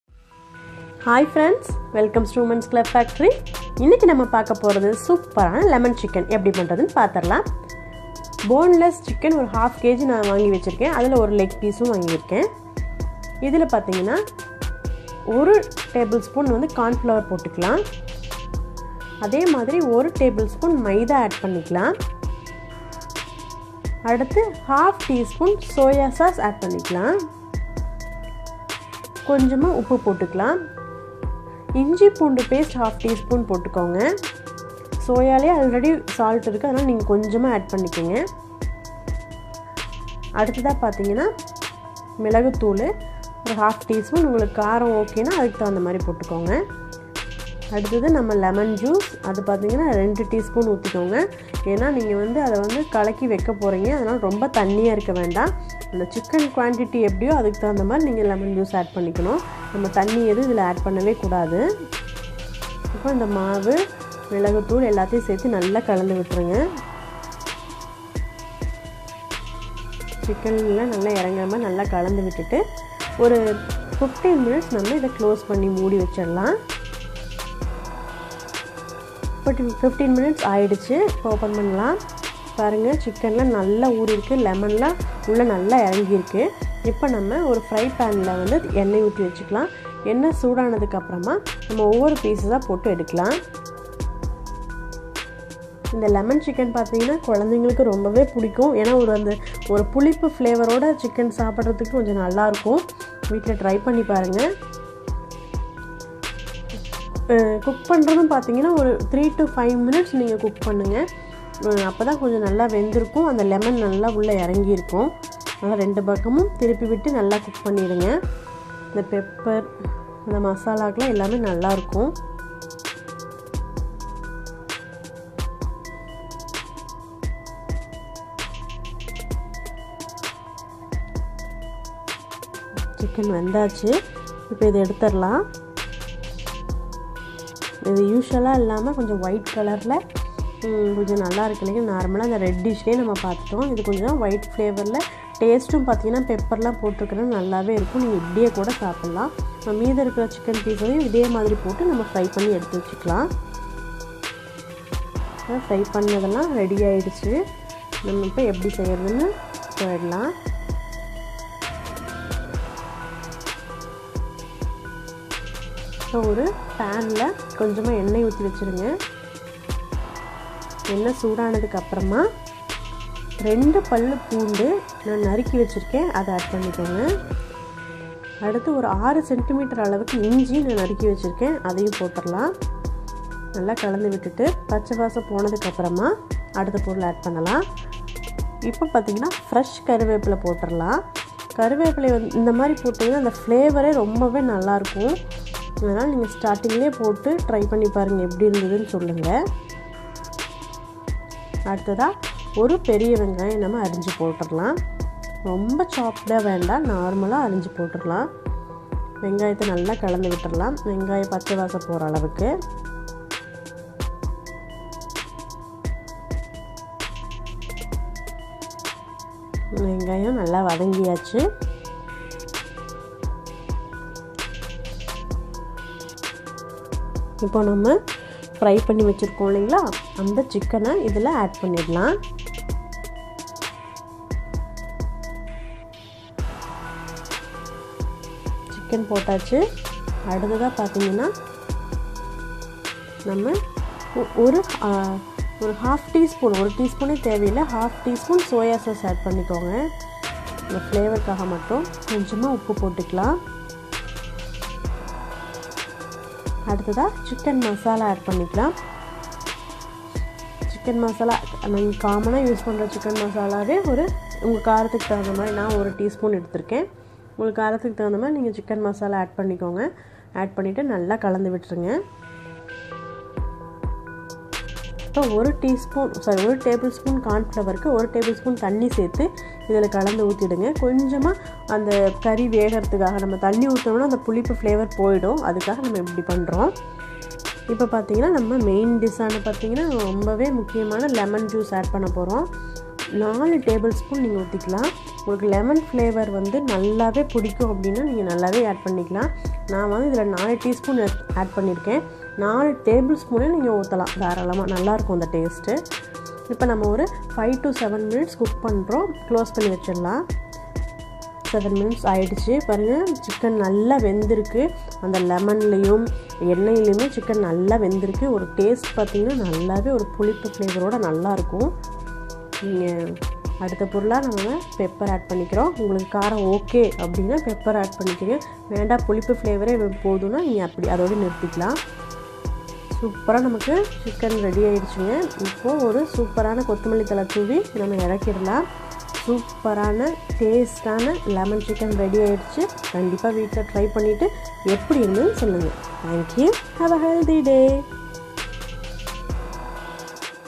Lemon chicken | How to make lemon chicken in tamil | Chicken Recipes
This video contains how to make simple and tasty lemon chicken in hotel style at home,in Tamil and also English Subtitle...
Keep watch and comments your experience...

WCF.